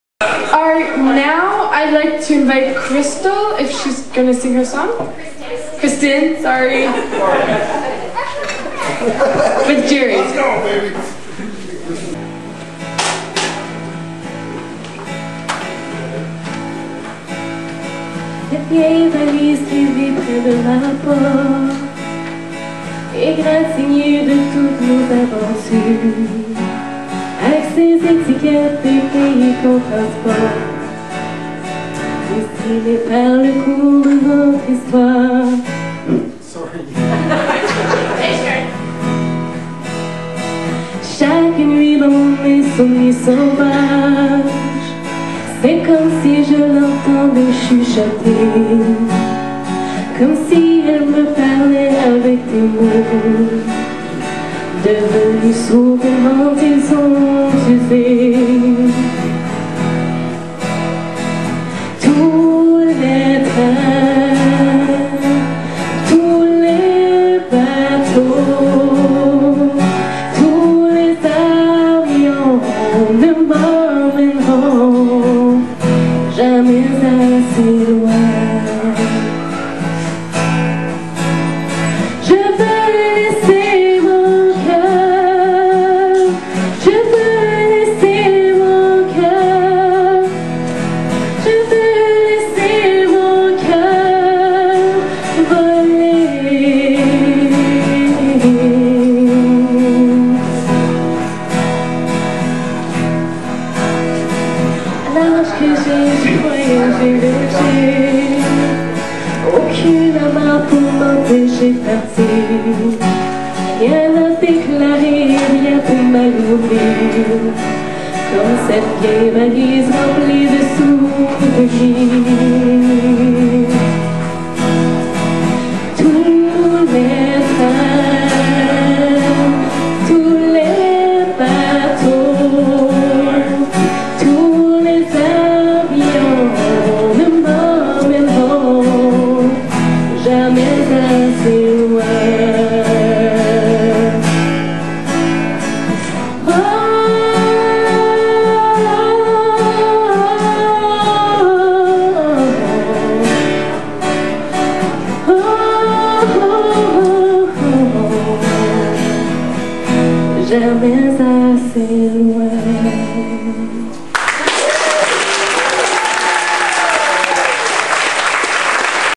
Krystin & Gerry-2010
Beautiful French song by Krystin & Gerry at the guitar
Reunion 2010